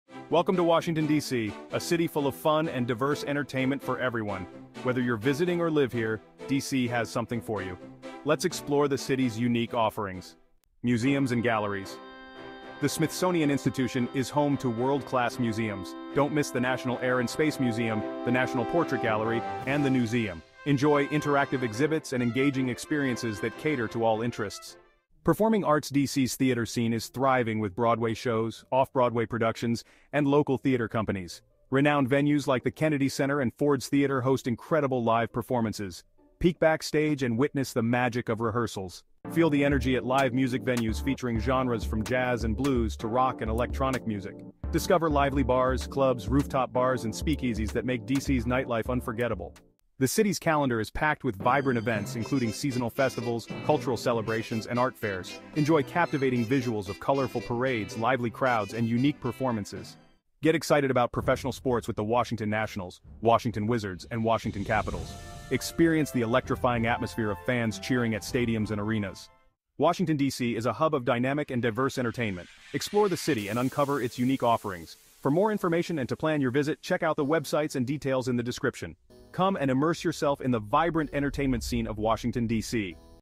Washington DC Entertainment
Experience the vibrant entertainment scene of Washington, D.C. with these top recommended tours and activities. 🌟🏛️

Discover the best of D.C. entertainment:

🌟 Explore Capitol Hill on a Guided Tour - Delve into the heart of American politics with an in-depth guided tour of Capitol Hill.

🌟 Washington D.C. Guided Tour Over Night by Vintage Cart - Enjoy a unique nighttime tour of the city in a charming vintage cart.

🌟 Guided Delicious Donut Tour with Tastings - Indulge in a sweet adventure with a guided donut tour featuring tastings of D.C.'s finest donuts.

🌟 Washington DC's Highest Rated Comedy Magic Show - Be amazed and entertained at D.C.'s top-rated comedy magic show.

🌟 National Gallery of Art - Guided Museum Tour - Explore world-class art collections with a guided tour of the National Gallery of Art.

Make the most of your visit to Washington, D.C. with these exciting entertainment options. 🎭🎨🍩